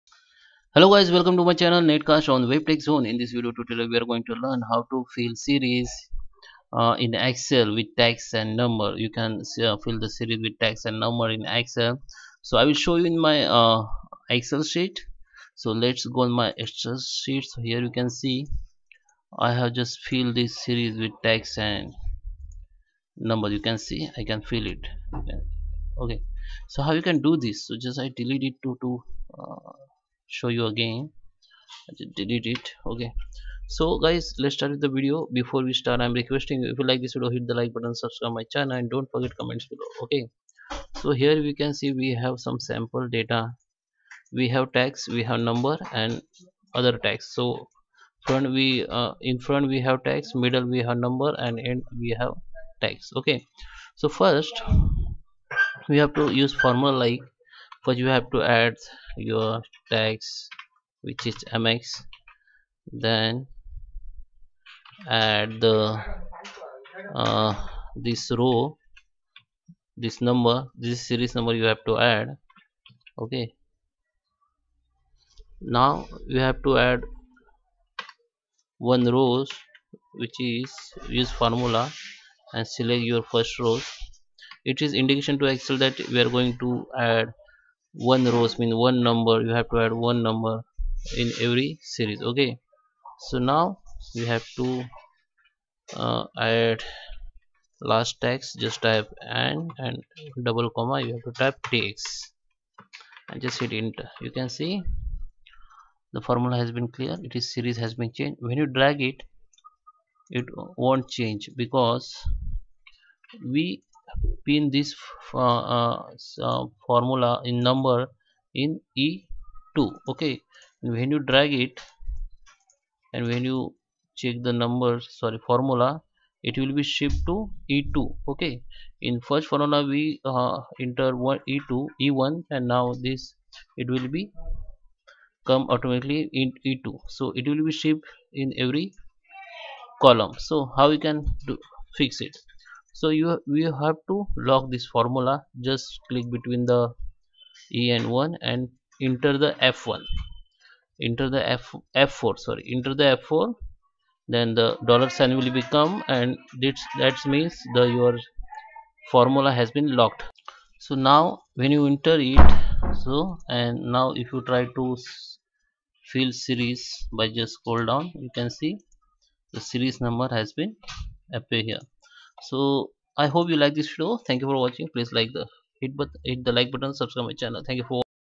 Excel Fill Series With Letters And Numbers
In this video tutorial, we are going to learn, how to fill series in excel with letters and numbers.